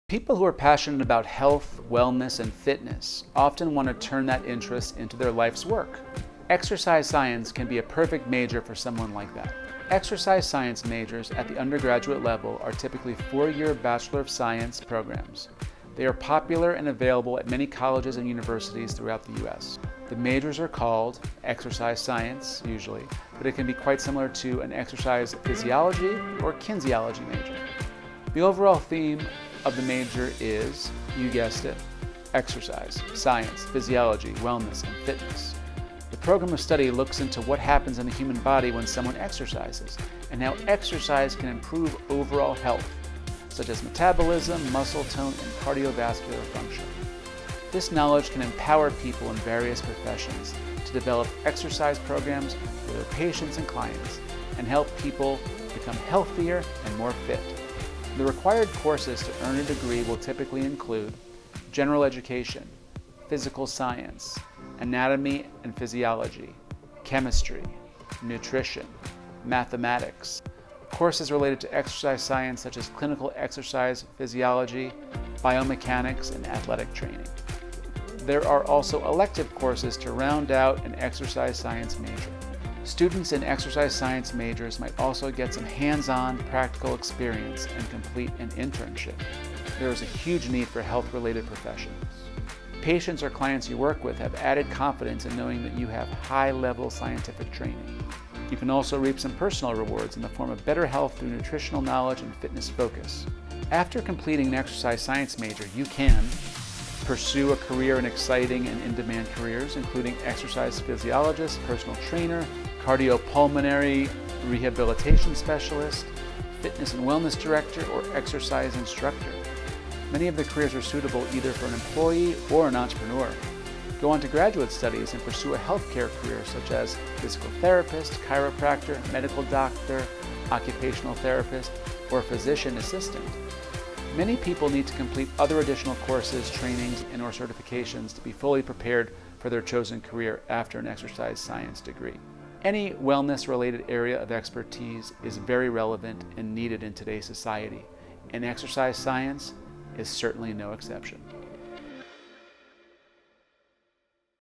All You Need To Know About an Exercise Science Major.
Exercise Science Majors

People who are passionate about health, wellness, and fitness often want to turn that interest into their life’s work. And exercise science can be a perfect major for someone like that.
Exercise science majors at the undergraduate level are typically 4-year bachelor of science (B.S) programs. They are popular and available at many colleges and universities throughout the U.S.

THE MAJORS ARE CALLED:
…exercise science, usually, but it can be quite similar to an exercise physiology or kinesiology major.

THE OVERALL THEME OF THE MAJORS IS:
…you guessed it, exercise, science, physiology, wellness, and fitness. The program of study looks into what happens in the human body when someone exercises, and how exercise can improve overall health, such as metabolism, muscle tone, and cardiovascular function. This knowledge can empower people in various professions to develop exercise programs for their patients and clients, and help people become healthier and fitter.

The required courses to earn a degree will typically include:
• general education
• physical science
• anatomy and physiology
• chemistry
• nutrition
• mathematics
• kinesiology
• courses related to exercise science, such as clinical exercise physiology, biomechanics, and athletic training
There are also elective courses to round out an exercise science major.
VIDEO NOTE: Could show the names of all courses on the screen as narrator is reading them.
Students in exercise science majors might also get some hands-on, practical experience and complete an internship.

SOME OF THE BENEFITS OF EXERCISE SCIENCE MAJORS:
1. One benefit is the ability to help other people be healthier.
2. You also gain in-demand knowledge and skills, and there is a huge need in health-related professions.
3. Patients or clients you work with have added confidence in knowing you have high-level scientific training.
4. You can also reap some personal rewards, in the form of better health through nutritional knowledge and fitness focus.

AFTER COMPLETING AN EXERCISE SCIENCE MAJOR, YOU CAN:
• Pursue a career in exciting and in-demand careers, including exercise physiologist, personal trainer, cardiopulmonary rehabilitation specialist, fitness and wellness director, or exercise instructor. Many of the careers are suitable either for an employee or an entrepreneur.
• Go on to graduate studies and pursue a healthcare career such as physical therapist, chiropractor, medical doctor, occupational therapist, or physician assistant. Many people need to complete other additional courses, trainings, and/or certifications to be fully prepared for their chosen career after an exercise science degree.
VIDEO NOTE: Maybe show some of the words from the items above on the screen while narrator is talking, and maybe present it as a numbered list.

Any wellness-related area of expertise is very relevant and needed in today’s society. And exercise science is certainly no exception.